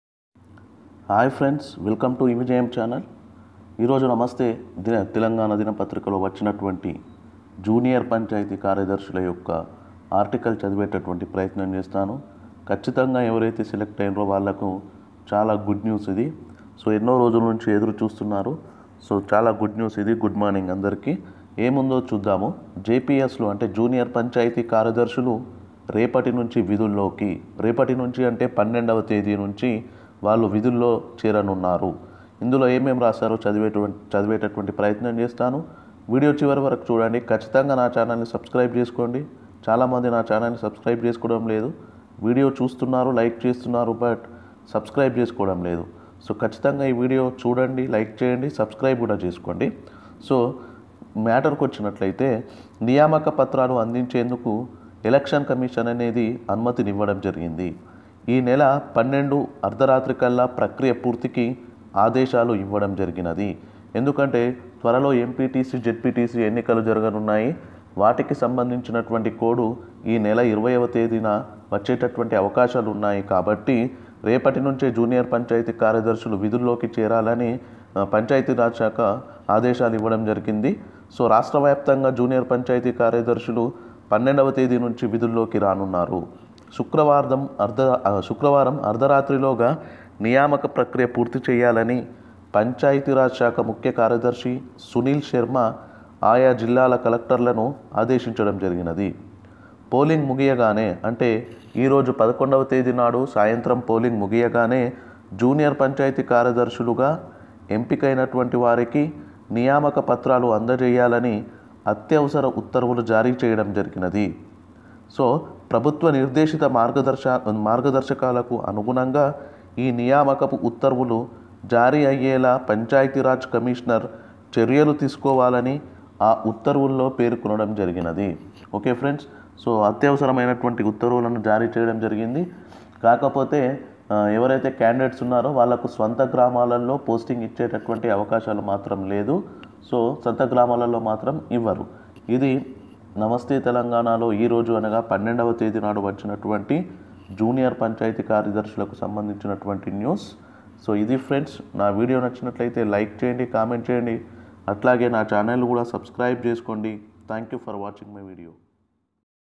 రేపటి నుండి (April 12) విధుల్లో కి పంచాయతి కార్యదర్శి లు.
Panchayathi department of telangana has given the orders to district collectors to give the appointment letters to candidates who were selected for junior panchayathi secretary post.